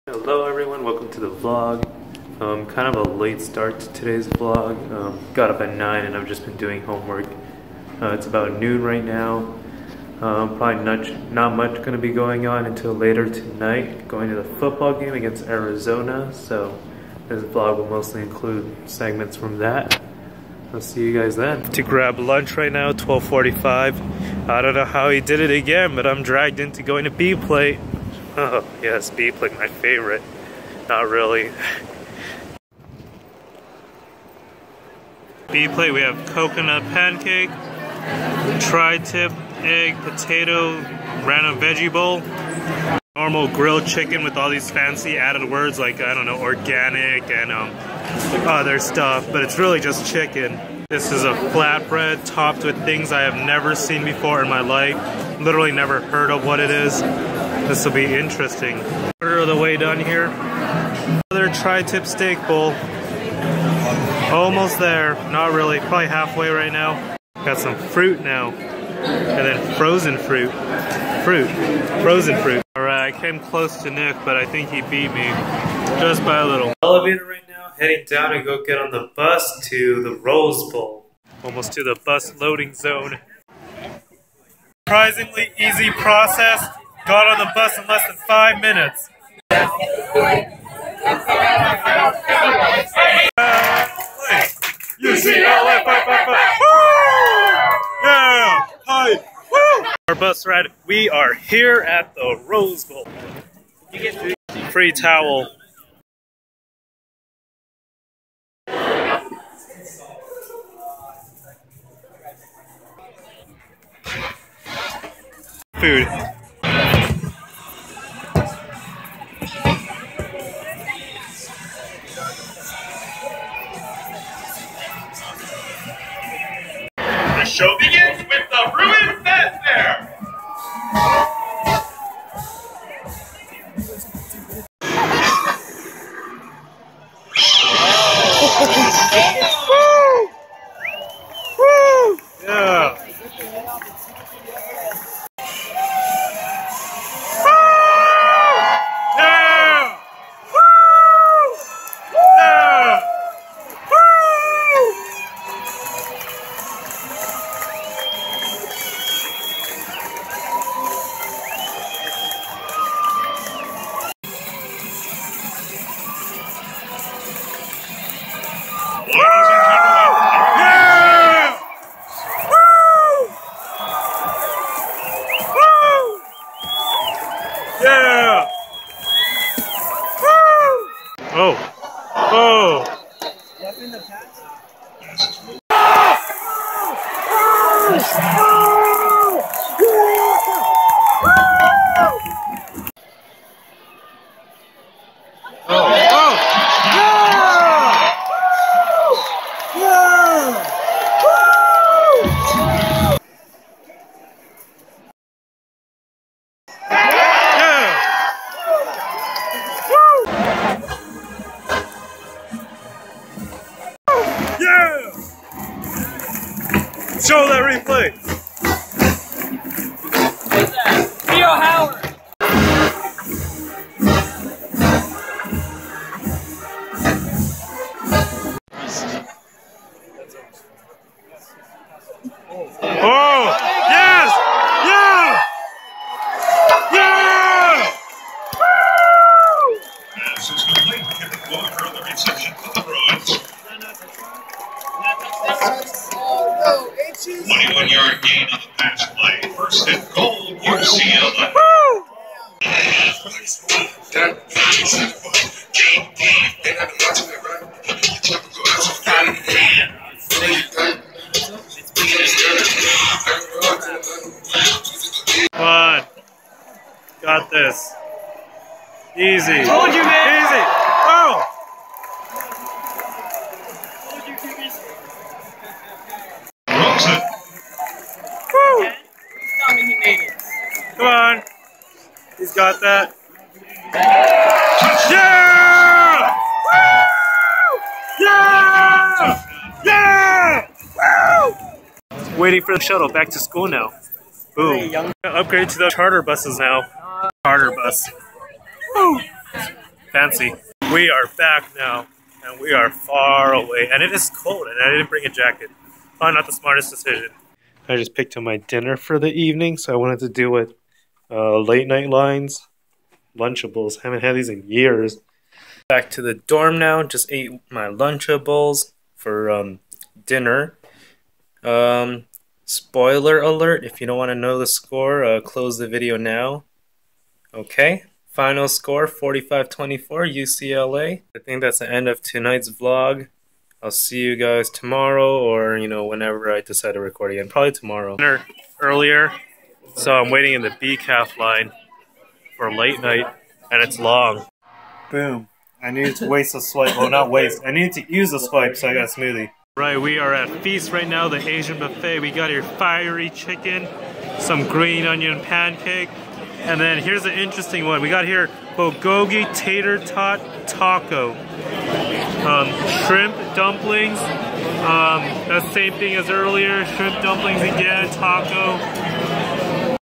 UCLA VS ARIZONA FOOTBALL 2016 | Vlog 048
Vlog 48
October 1, 2016
Pasadena, CA

In today's vlog, we have a late start to the day, simply grabbing lunch and later on by going to the UCLA vs Arizona football game at the Rose Bowl in Pasadena. Final score 45-24 UCLA